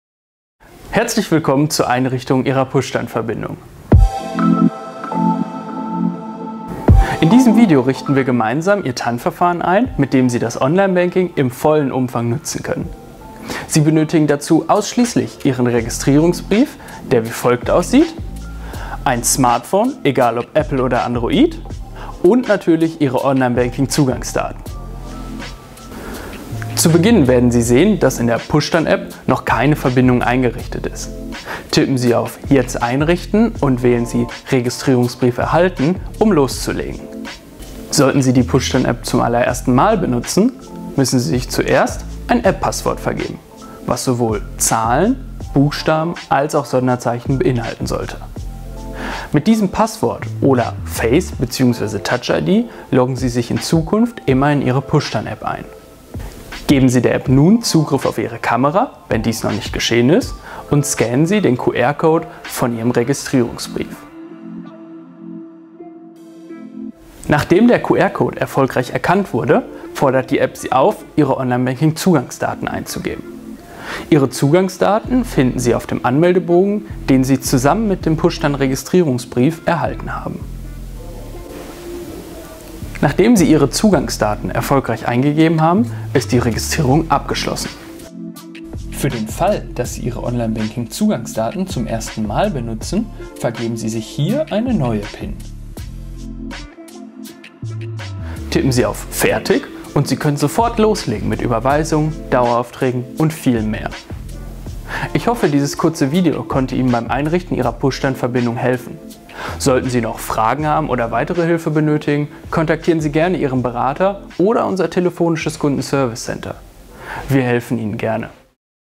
PushTAN-App einrichten | Sparkasse OnlineBanking
In diesem Video zeigen wir Dir, wie Du deine Push-TAN-Verbindung an deinem Handy freischalten kannst.
Du benötigst die Push-TAN-App, um im Online Banking Überweisungen zu tätigen, Daueraufträge einzurichten oder deine Umsätze abzurufen.
Im Video wird Dir erklärt, dass du einen PushTAN-Registrierungsbrief brauchst, um die Freischaltung abzuschließen. Diesen Registrierungsbrief bekommst Du über einen der folgenden Wege:
- Er wird bei der erstmaligen Eröffnung eines Online Banking-Vertrages in der Sparkasse mit ausgehändigt.
- In der Sparkasse kannst Du dir jederzeit einen neuen Registrierungsbrief erstellen lassen.
- Du kannst im telefonischen KundenServiceCenter einen neuen Registrierungsbrief beantragen. Dieser wird Dir dann im Anschluss per Post zugeschickt.
- Du kannst im Online Banking (unter Service Center) eine neue Registrierung beantragen. Der Registrierungsbrief kommt dann innerhalb weniger Tage per Post nach Hause.

Solltest Du dazu noch Fragen haben, melde Dich gerne bei uns unter einem der folgenden Wege: